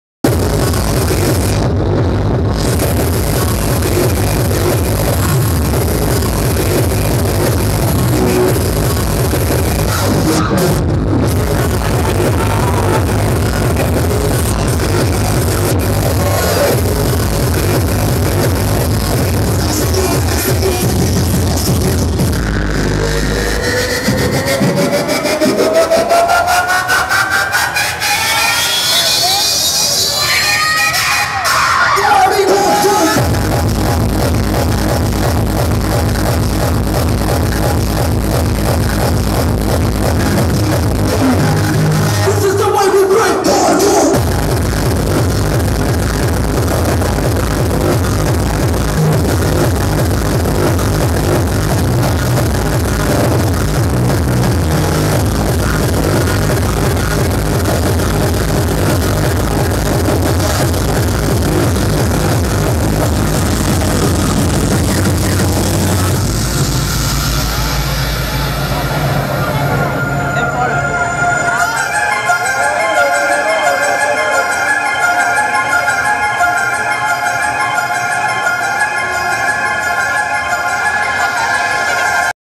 The Stunned Guys 10 years of Hardcore Italia Eleven Vallgroguina 3/3/2018
un vídeo de la sesión de The Stunned Guys del festival 10 years of Hardcore Italia en la sala macro de la discoteca Eleven de Vallgorguina